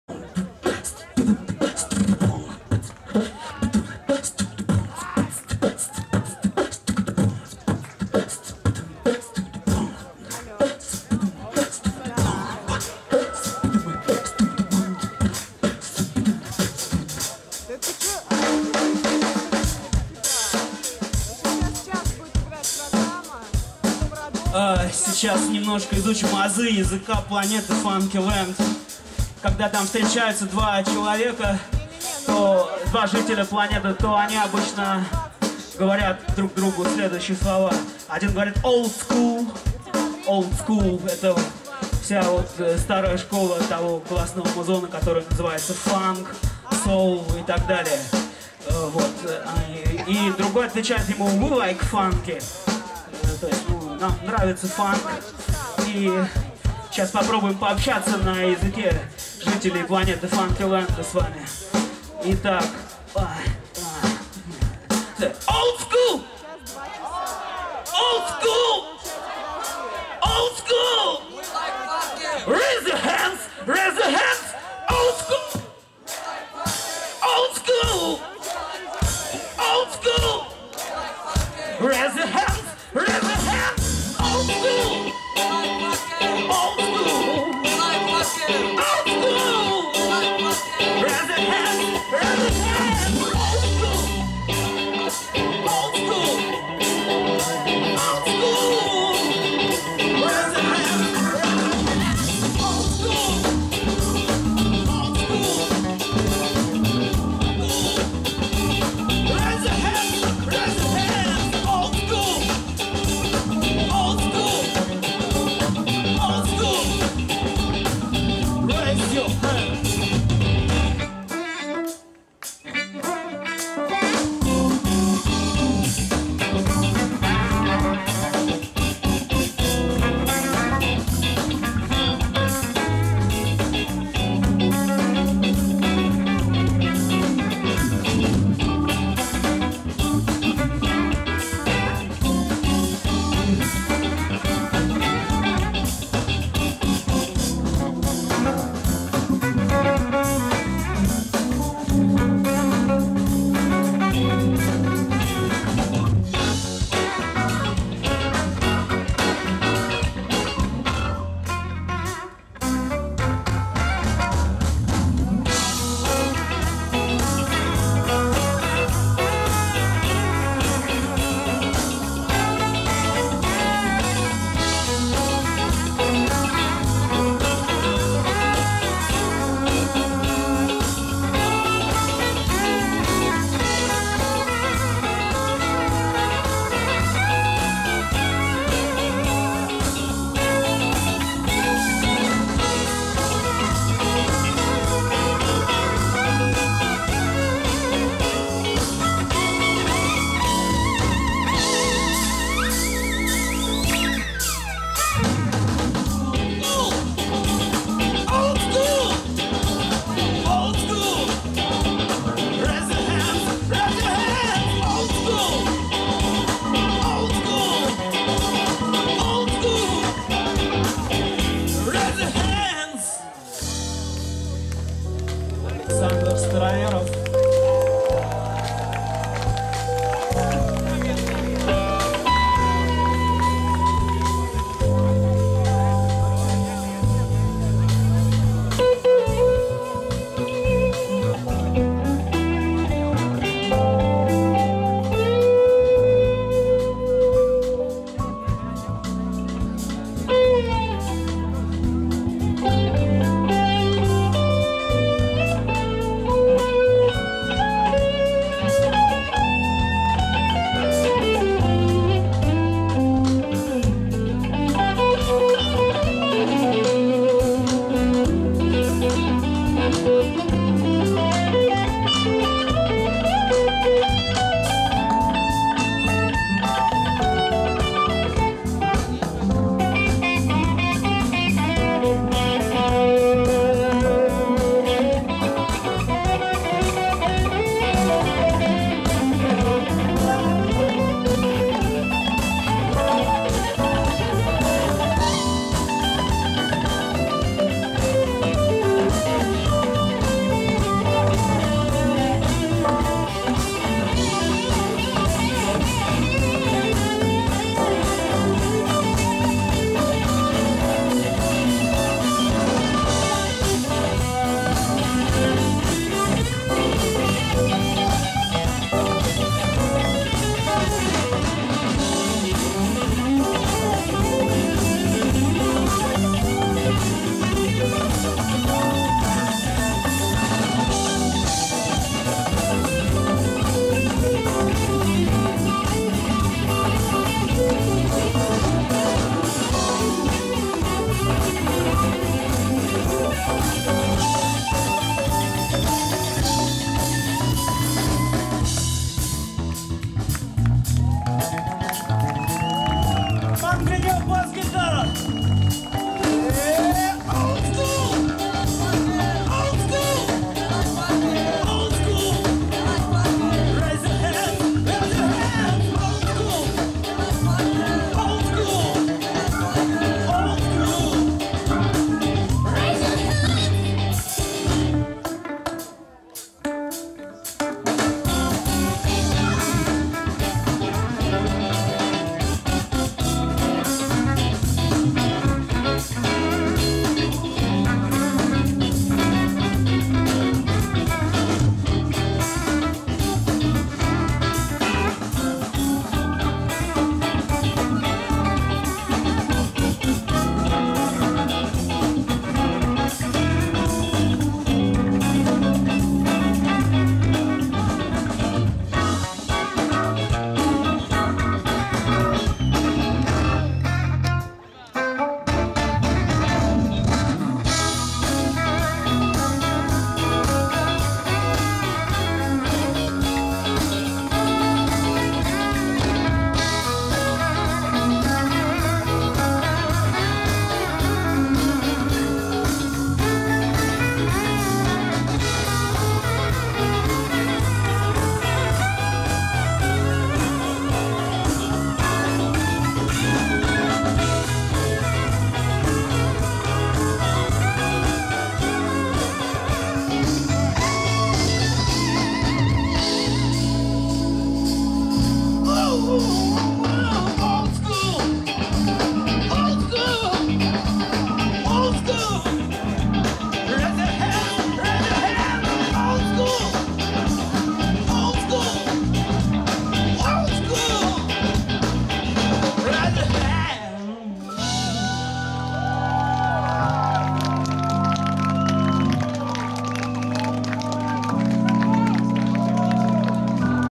Felix Lahuti & Funky Land in "Grushinskiy" festival ( project "Ostrov" ) summer 2010
Felix Lahuti - 5-strings electric violin
Dmitriy Ilugdin -keyboards
Alexandr Staroverov - guitar
Roman Grinev - bass
Anton Dashkin - drums